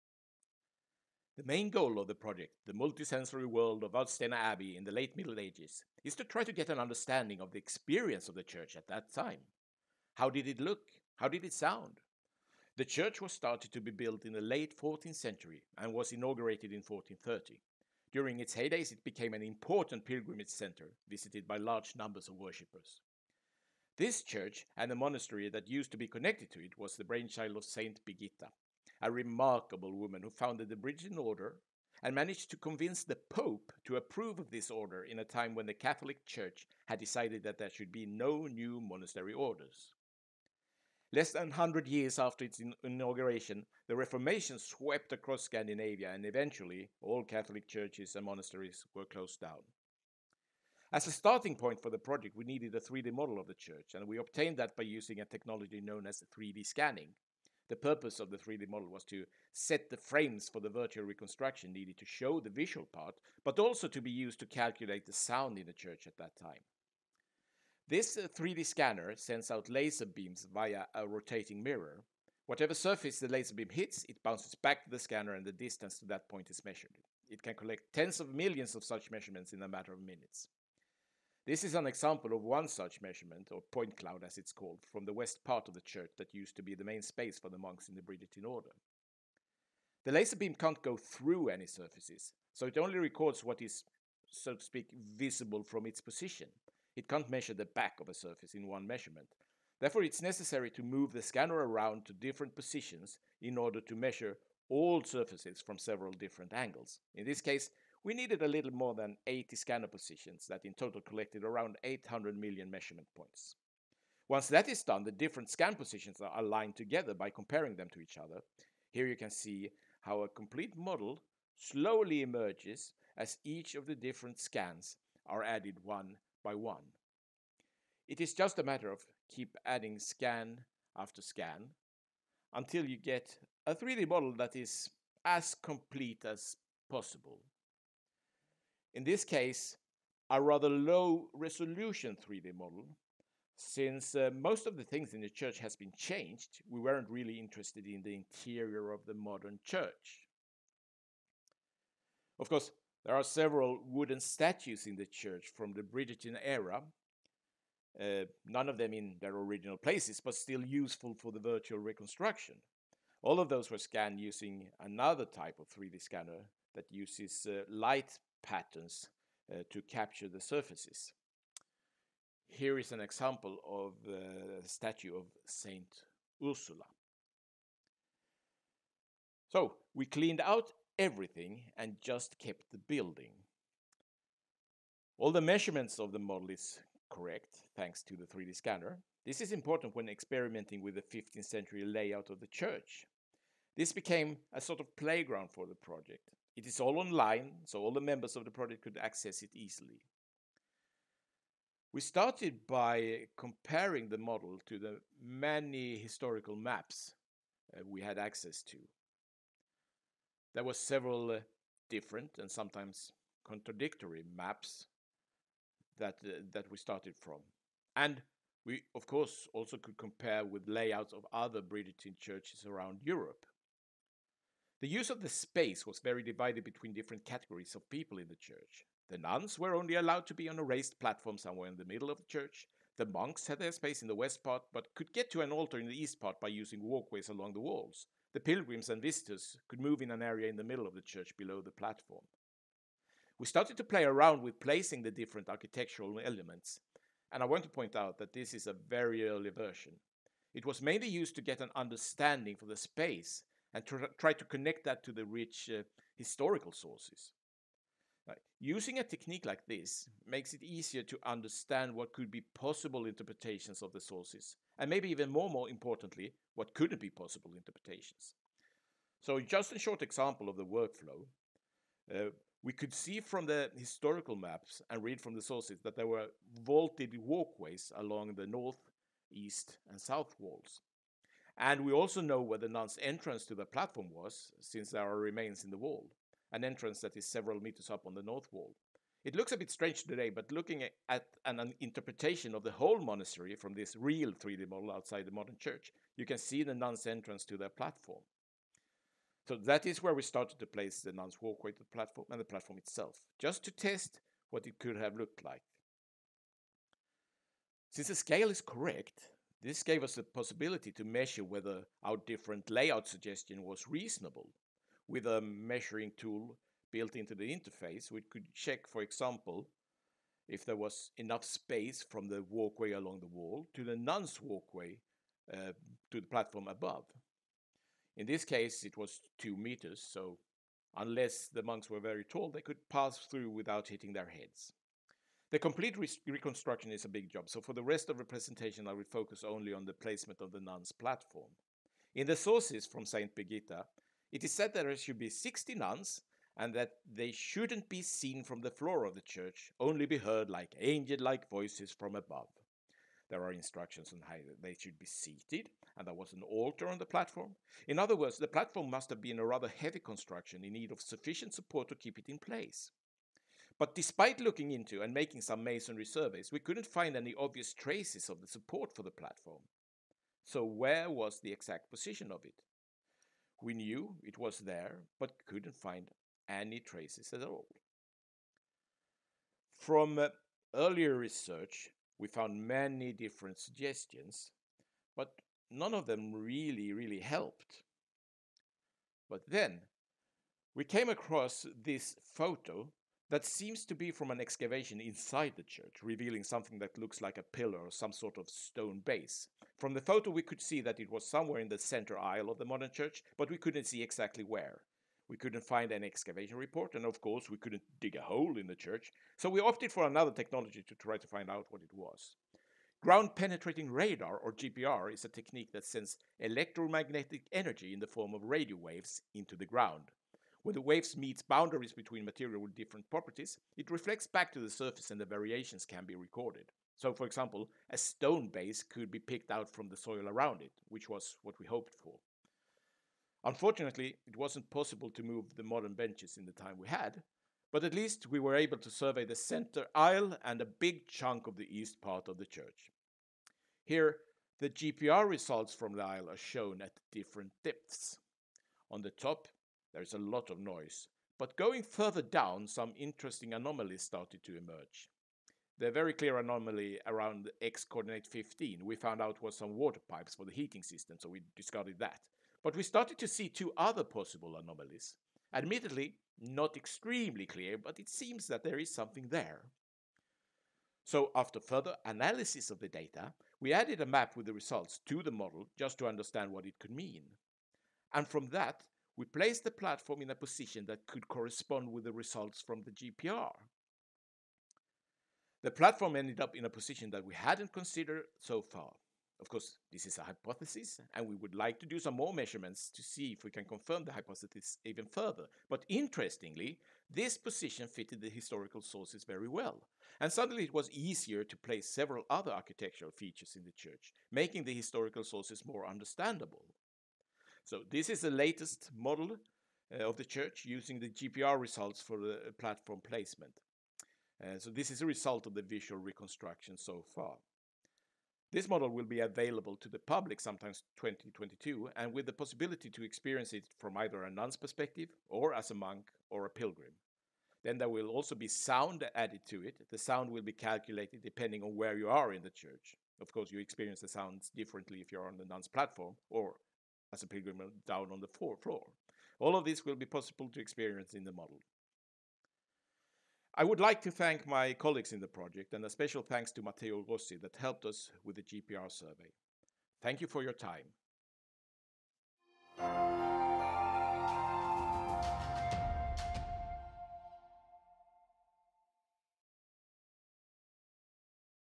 Groundbreaking insights without breaking ground
Short, recorded lecture on virtual reconstruction of Vadstena Abbey, Sweden

Presenter: Stefan Lindgren, Research engineer in Lund University Humanities Lab

The construction of Vadstena Abbey church started in the late 14th century and it was inaugurated in 1430. It was built following surprisingly detailed instructions from Saint Birgitta. Today the interior of the church looks very different from what it did in its glory days – since then it has gone through the reformation, several hundred years of neglect and thereafter the restorations in the early and late 19th century. But what did it look like back in the days? How was the sound in the church? And how did it smell? These are some of the questions that the project Multisensory Vadstena is trying to answer by creating a virtual reconstruction of the early church.

This presentation covers a small but significant part of this virtual reconstruction, namely the long debated placement of the nuns’ platform in the church. The presentation shows how we, with the help of modern technology like 3D-scanning and ground penetrating radar, in combination with written sources, have managed to come up with a new hypothesis that fits well with these sources.

Project members:
Eva Lindqvist Sandgren, Department of Art History, Uppsala University
Karin Strinnholm Lagergren, Department of Music and Art, Linnaeus University
Delphine Bard, Engineering Acoustics, Faculty of Engineering, Lund University
Hanna Autio, Engineering Acoustics, Faculty of Engineering, Lund University
Mathias Barbagallo, Engineering Acoustics, Faculty of Engineering, Lund University
Carolina Ask, 3D-consultant
Stefan Lindgren, Lund University Humanities lab

Other collaborators:
Matteo Rossi, Engineering Geology, Faculty of Engineering, Lund University